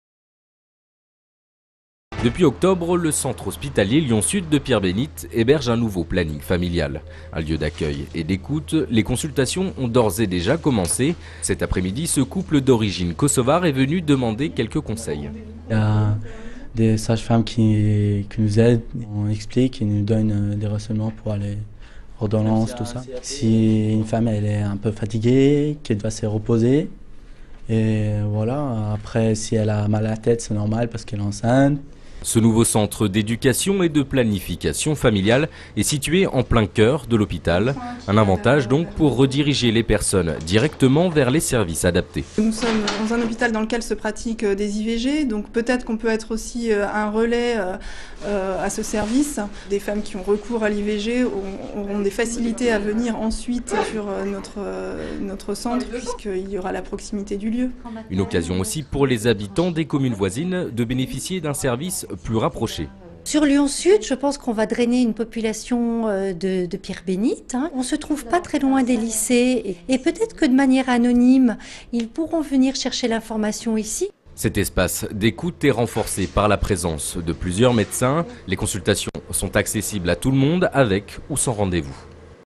Lyon-Sud: un nouveau planning familial (Pierre-Bénite)
Vous voulez conserver ce reportage ?
 Un centre de planification et d'éducation familiale vient d'ouvrir à l'hôpital Lyon-Sud, à Pierre-Bénite.